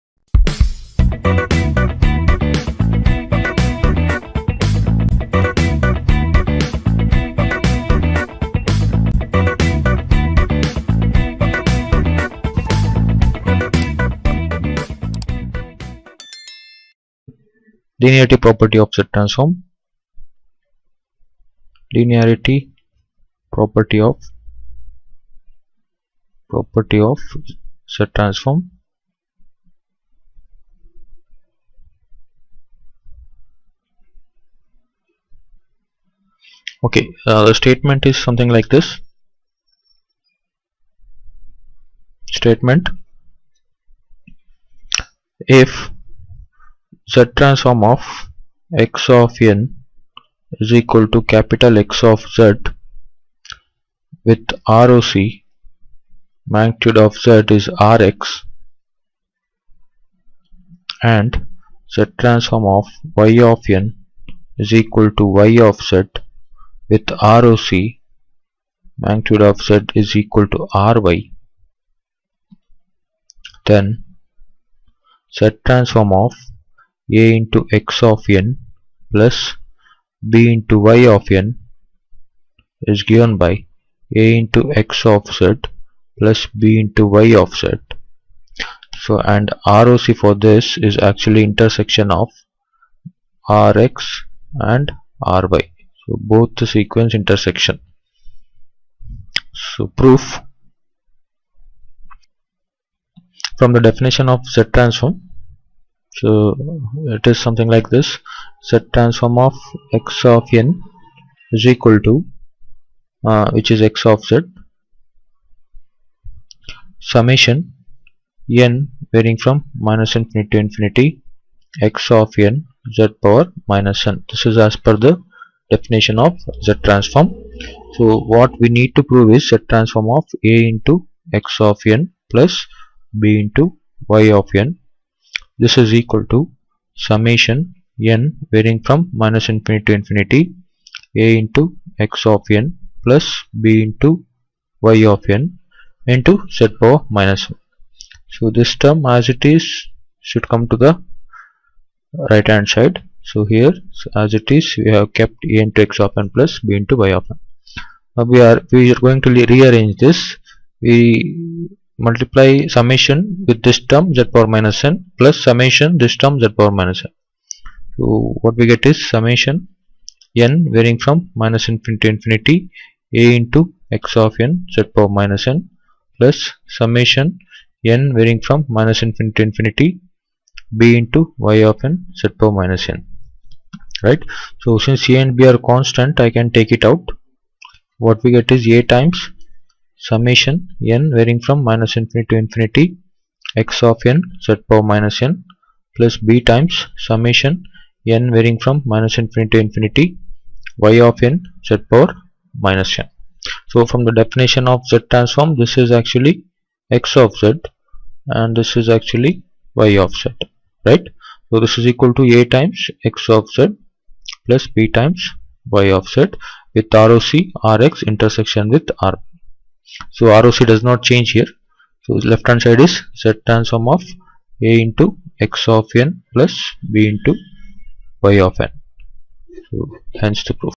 VTU-SIGNALS AND SYSTEMS (18EC45)-M5-L11-TO PROVE LINEARITY PROPERTY OF Z TRANSFORM
In this video, i have tried to prove linearity property of Z Transform
faculty name: Mr. Vayusutha M

Asst. Professor, Dept. of ECE, Canara Engineering College